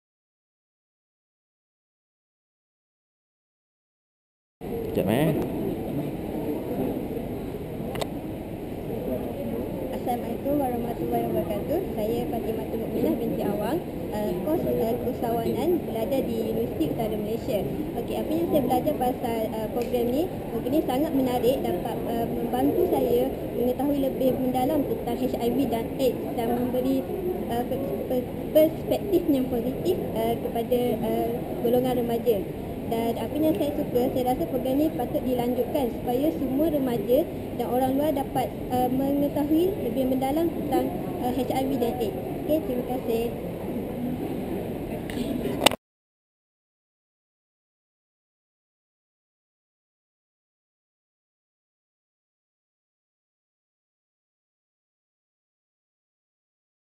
Red Ribbon Youth Club Workshop Key Learning Points
Fatimahtul Mukminah of UUM shares her key learning points on the RRYC Workshop in UUM. RRYC is an effort to reverse the negative impact of HIV and AIDS among the youth segment in Malaysia. This is a join effort by the Red Ribbon Celebrity Supporter Aaron Aziz and Malaysian AIDS Council.